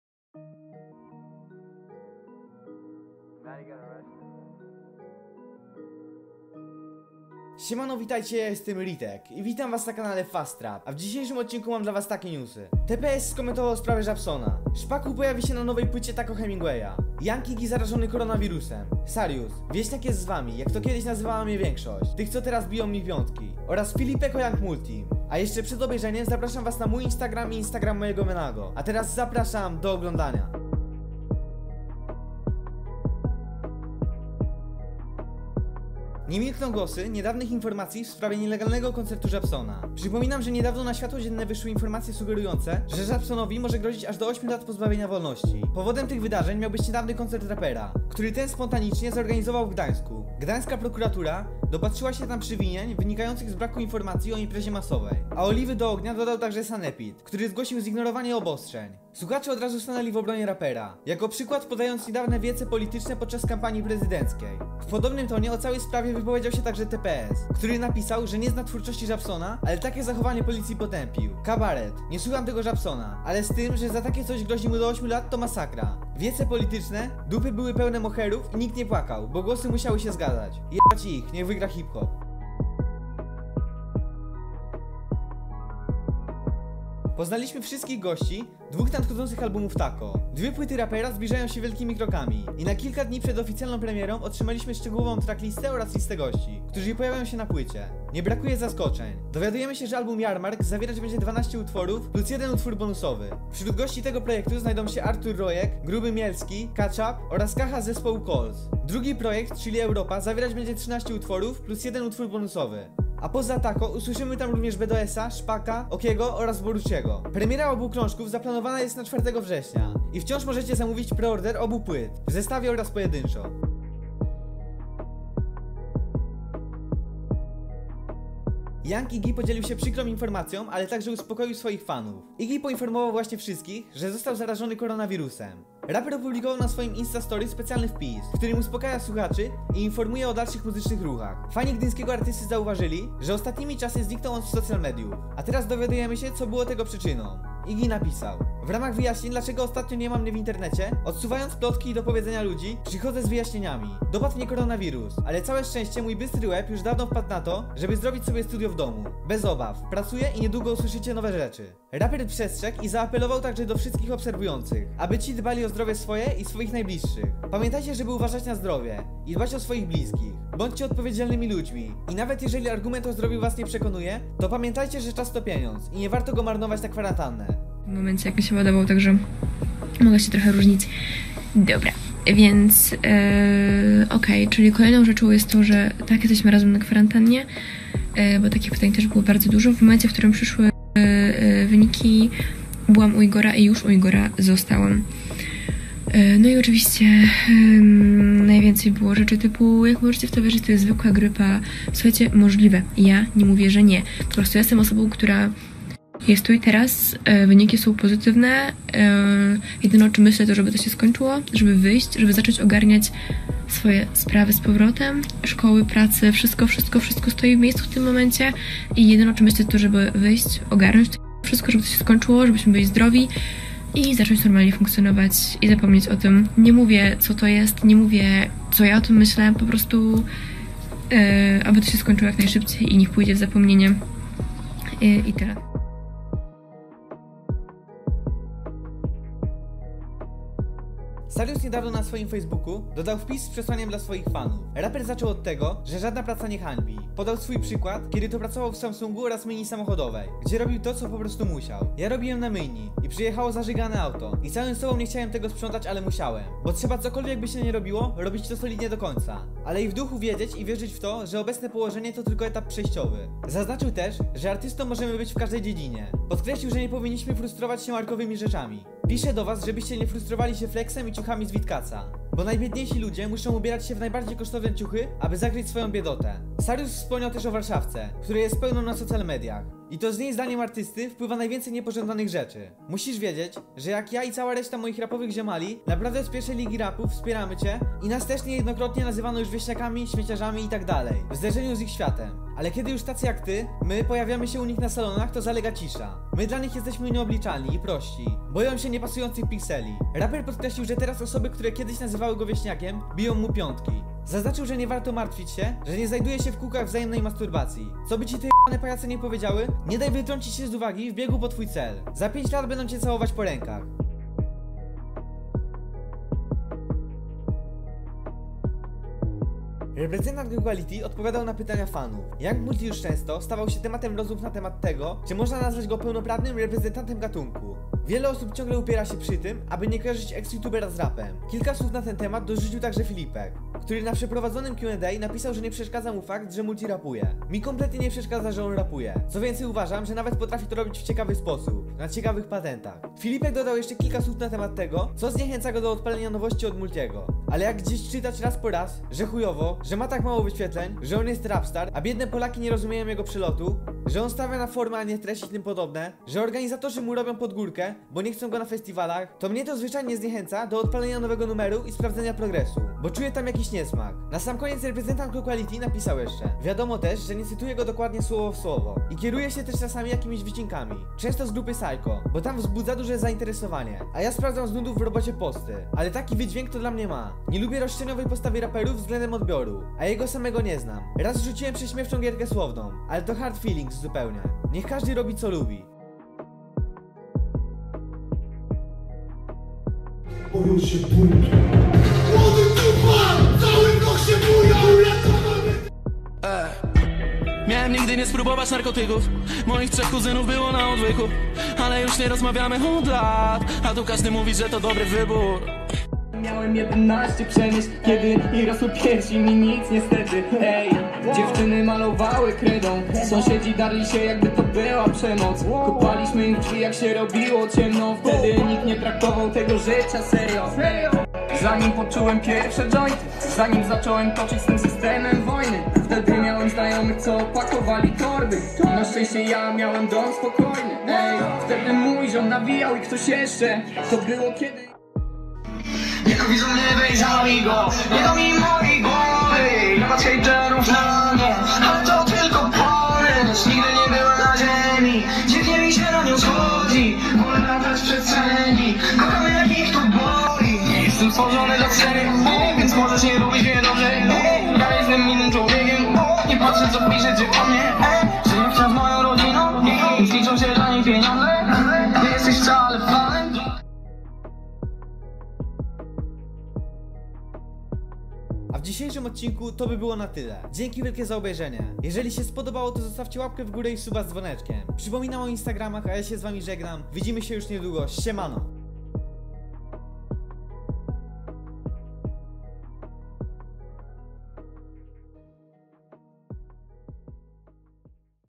SARIUS OSTRO O HEJTERACH, YOUNG IGI ZARAŻONY, TPS POPIERA ŻABSONA 😱
! PIOSENKA DNIA ! - ???
🔥 MÓJ INSTAGRAM - y_ritek
☑ UDOSTĘPNIJ FILM = WIĘKSZY ROZGŁOS! ☑
👑 WIDZIMY SIĘ NIEDŁUGO 👑